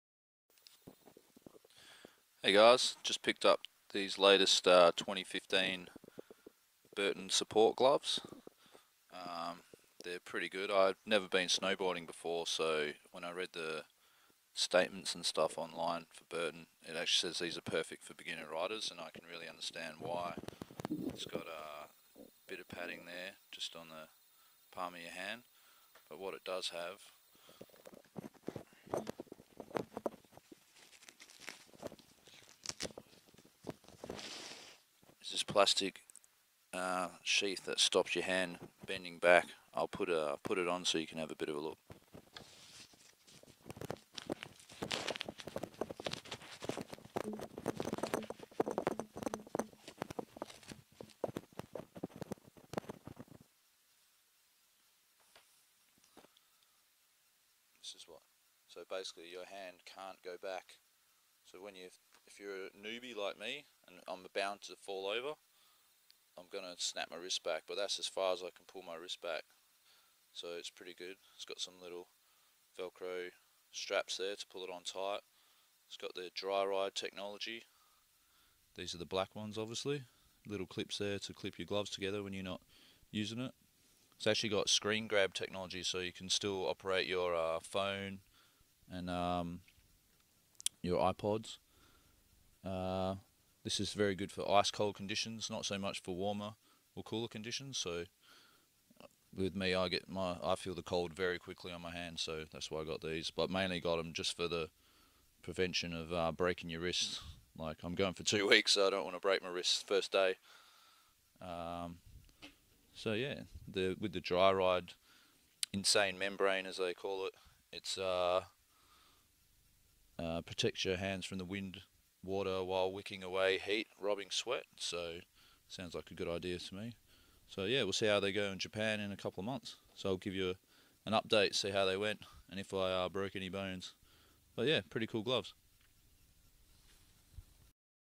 BURTON SUPPORT GLOVE 2015
Whether you’re learning to turn or landing your first triple backflip, there is a possibility you may break a bone. Casts are cool, but the three-hour wait in the emergency room should be reason enough to get the Burton Support Glove. Combining everything you could ever want in a normal waterproof glove, it also secretly protects thanks to a low-profile, ergonomic internal support system. Fully loaded, the Support Glove combines DRYRIDE Ultrashell fabric and Insane Membrane technology for a double dose of waterproof/breathability. Screen Grab magic gives you stealth gloves-on touchscreen control with all ten fingers.

Screen Grab Toughgrip Palm for Total Touchscreen Control
Low-Profile, Removable Internal Support System
Ergonomic, Articulated Left and Right Fit
DRYRIDE Ultrashell 2-Layer Fabric
Waterproof DRYRIDE Insane Membrane 2.0
Thermacore Insulation
Brushed Microfiber Fixed Lining
Ergonomic Pre-Curved Fit
Unisex Sizing

Material: DRYRIDE Ultrashell 2-Layer Fabric
Recommended Use: Snow Sports
Insulation: Thermacore Insulation
Removable Liner: Low-Profile, Removable Internal Support System